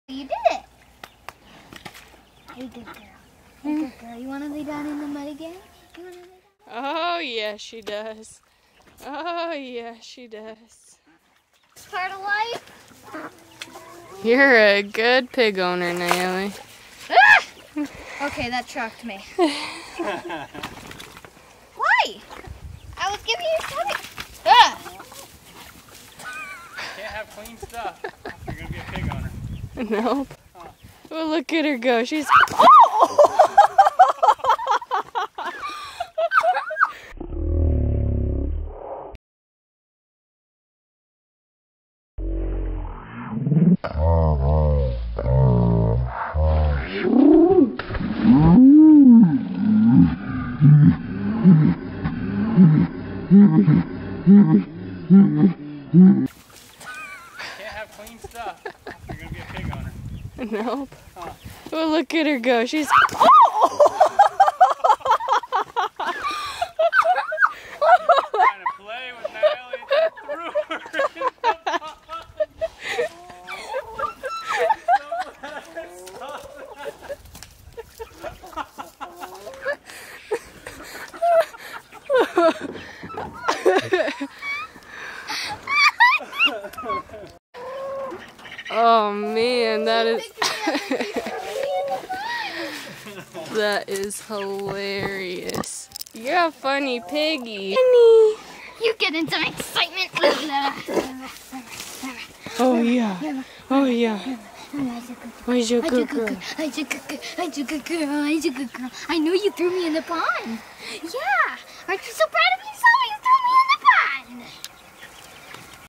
Honey the pig throws Nayeli into the Pond!
Our sweet Honey pig has been spoiled with belly rubs all summer long. Watch what happens when she is in a playful mood!